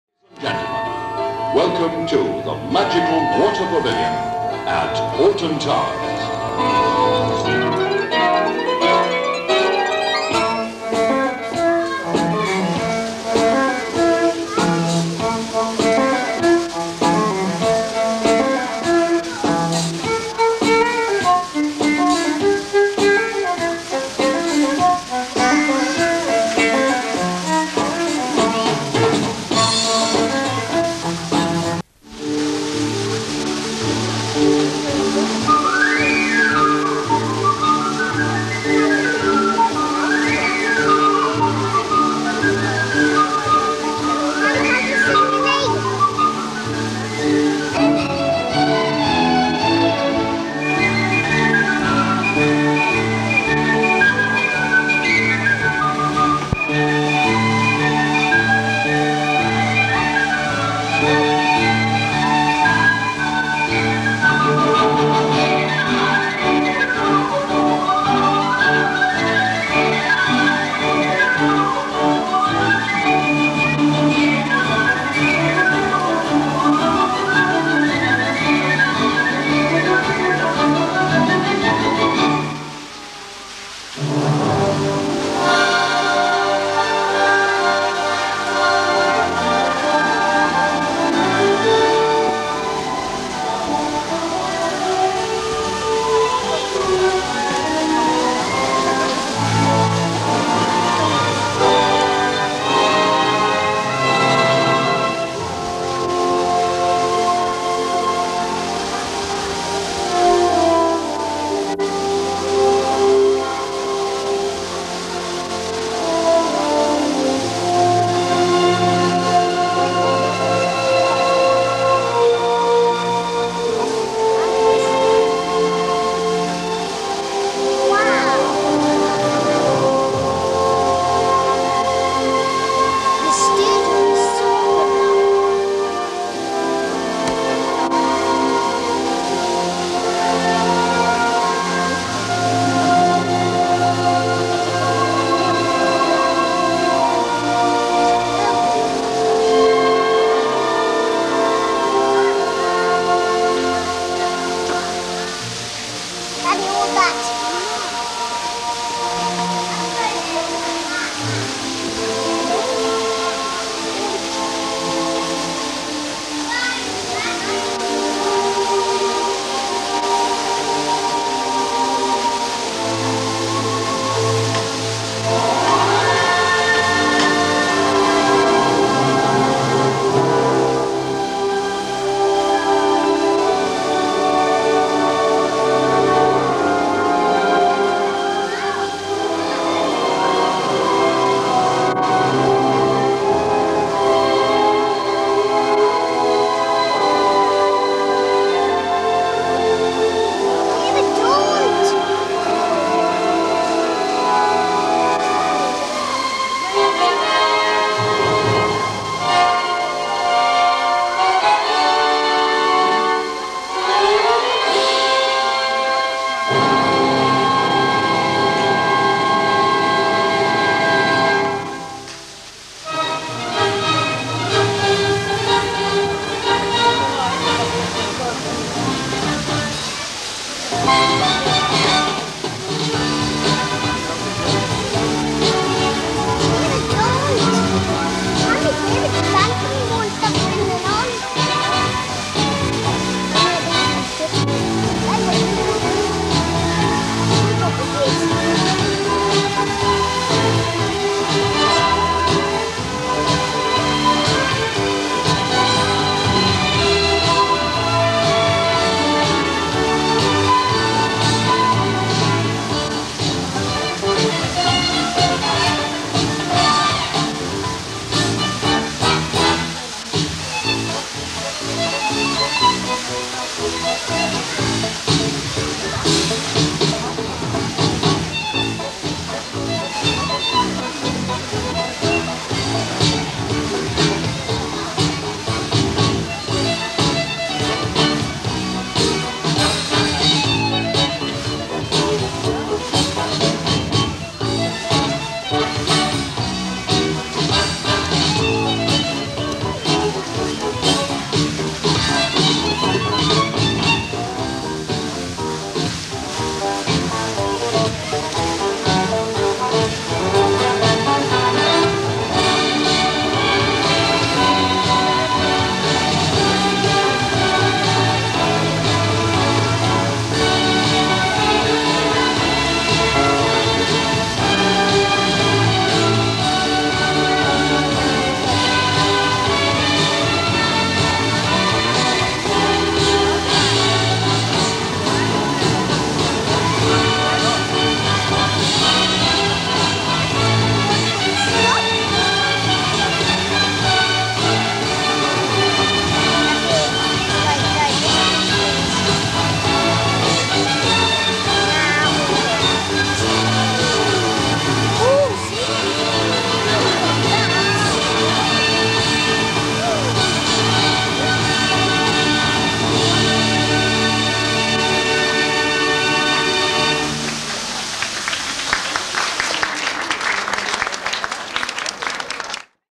The Magical Water Pavilion ( Alton Towers ) - ( 1988 )
In the 80s and 90s my dad liked to film absolutely everything on what I remember was a Philips vkr 6820 (VHS) - (he was the original family vlogger lol.)

Among the many places we liked to visit in the 80s and 90s were theme parks like Alton Towers, Flamingo Land, Lightwater Valley etc.. and as I am in the process of digitising these old VHS tapes, I thought I would be interesting to post footage featuring old rides at these Theme Parks that no longer exist but people like myself have very fond memories of.

For my second video looking back at old Theme Park footage I thought I would post "The Magical Water Pavilion" at Alton Towers in 1988.As far as I know this is rare footage because there are no other videos of it online that I could find.

There are some small jumps at the beginning that I had to edit because my Dad reached the end of his tape at 25 seconds into the show and had to put in a fresh one (in the dark haha), but this is pretty much the whole show.

This is a really cool show that I would probably still enjoy if I was to see it today and its one I always remember really well. I don't know much about where this was located at Alton Towers or when it closed but one little thing I do know (thanks to You Tube's checks) is that some or perhaps all of the music was composed by Andrew Lloyd Webber.

The little voices you hear in the background are me and my older brother :)